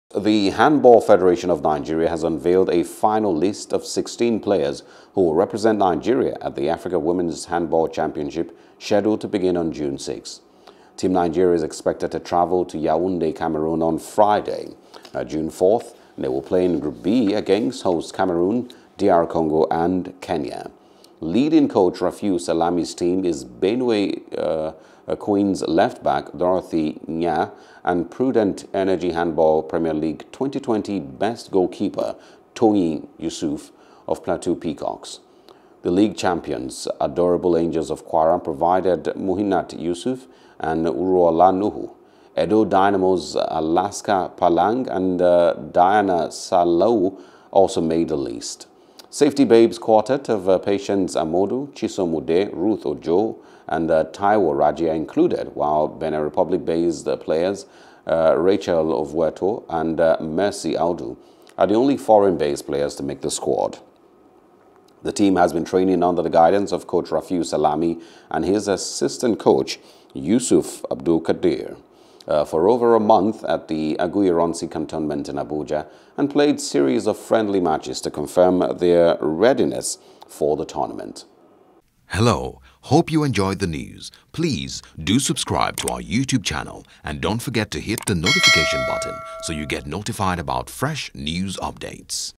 Handball: Nigeria Unveils Final Team List For Africa Women’s Nations Cup | SPORTS
The Handball Federation of Nigeria has unveiled a final list of sixteen players who will represent Nigeria at the Africa Women’s Handball Championship scheduled to begin on June 6.

Team Nigeria is expected to travel to Yaoundé, Cameroon on Friday, June 4. They will play in group B against host Cameroon, DR Congo, and Kenya.